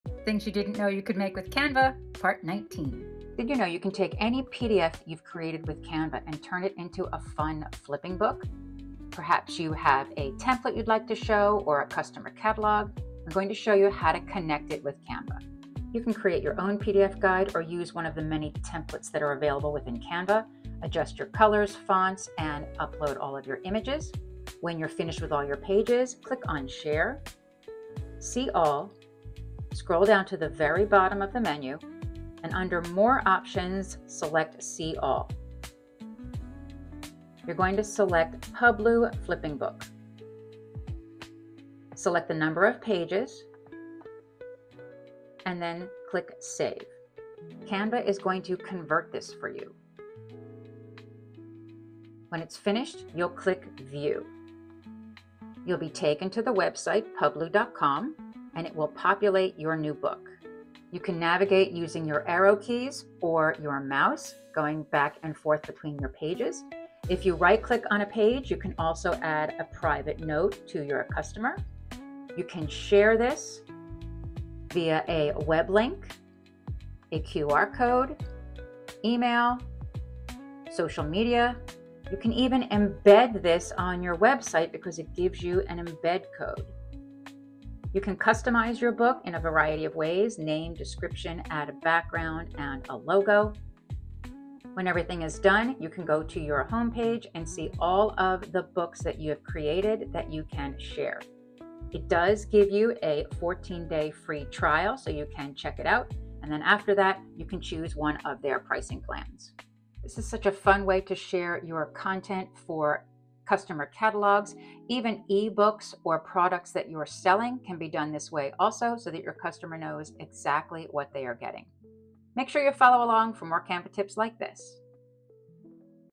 Create a flipping book with Canva!
📖 Did you know you can turn any Canva PDF into a flipping book?

Turning your Canva PDFs into interactive flipping books is super easy in just a few clicks! ✨

Whether it’s a product catalog, customer guide, or even an ebook, Canva integrates with Publuu Flipping Book to make your files look polished and professional. You can share your book as a link, embed it on your website, or even generate a QR code.

It’s a creative way to showcase your content, wow your customers, and give your designs that extra “pro” touch.

Would you use this for a catalog or an ebook? 👇

🩵Follow for more Canva hacks you didn’t know existed!